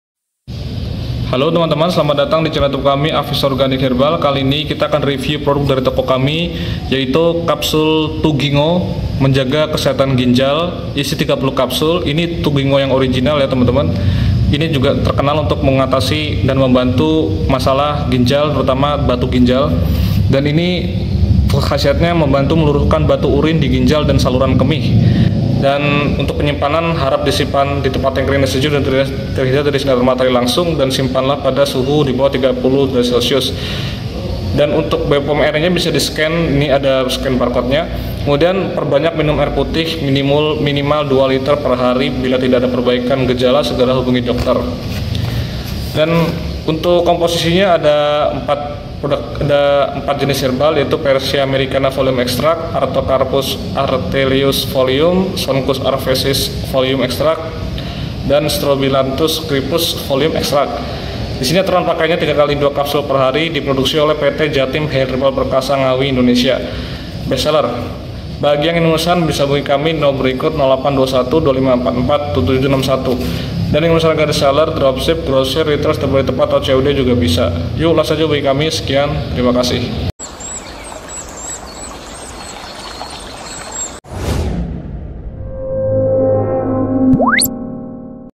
REVIEW TUGINGO Obat Penghancur BATU URIN
TuginGo merupakan herbal alami yang secara klinis teruji dapat membantu meluruhkan batu urin di ginjal dan saluran kemih.  TuginGo memiliki kandungan utama Persea americana yaitu buah alpukat, dimana buah alpukat tinggi akan kandungan potassium yang mampu menurunkan tekanan darah. Sebab pada dasarnya potassium dapat membantu memperluas pembuluh darah. Kandungan potassium alpukat juga membantu ekskresi kalsium urin dan menurunkan risiko batu terbentuk di ginjal. Manfaat lain dari TuginGo juga dapat membantu mengatasi berbagai keluhan pada organ ginjal seperti infeksi ginjal, batu ginjal, gagal ginjal akut, nefropati diabetik, sindrom nefritik, sindrom nefrotik, dan lain sebagainya.

KHASIAT & MANFAAT :
1.Membantu meluruhkan batu urin di ginjal dan saluran kemih.
2.Mengobati keluhan infeksi ginjal.
3.Mengobati batu ginjal.
4.Mengobati gagal ginjal akut.
5.Mengobati neropati diabetik (penyakit ginjal yang disebabkan oleh diabetes)
6.Mengobati sindrom nefritik (sekumpulan gejala yang terjadi sebagai akibat dari peradangan ginjal)
7.Mengobati sindrom nefrotik (kerusakan pada ginjal yang menyebabkan kadar protein di dalam urine meningkat)
8.Mengandung berbagai herbal alami yang baik untuk menjaga kesehatan ginjal.
9.Mengobati Hipertensi atau tekanan darah tinggi.
10.Sebagai antioksidan sehingga dapat menangkal radikal bebas.
11.Sebagai antitumor dan antikanker.

Komposisi
🌿Persea americana 200mg = alpukat
🌿Artocarpus altilis 100mg    = sukun
🌿sonchus arvensis 100mg = tempuyung
🌿strobilanthes crispus 100gr = keci beling

Cara Konsumsi
Sehari 3x2kapsul untuk penderita ginjal akut
Sehari 3x1kapsul untuk kaka yang dinyatakan gejala penyakit ginjal
Dapat juga digunakan sebagai suplemen untuk menjaga kesehatan ginjal

Anjuran: Imbangi dengan konsumsi air putih min 2lt/hari

Cara Penyimpanan
Simpan ditempat yang kering dan terhindar dari paparan sinar matahari secara langsung, simpan dibawah suhu 30°C
Isi Kemasan : 30 Caps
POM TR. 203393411
Diproduksi oleh : PT Jatim Herbal Perkasa Ngawi - Indonesia
AVISTORES Organic Herbal
☎️ Call Center :
📕 Facebook Pages : AVISTORES Organic Herbal
🛒 Marketplaces :
🛍 Shopee-Bukalapak-Tokopedia : AVISTORES Organic Herbal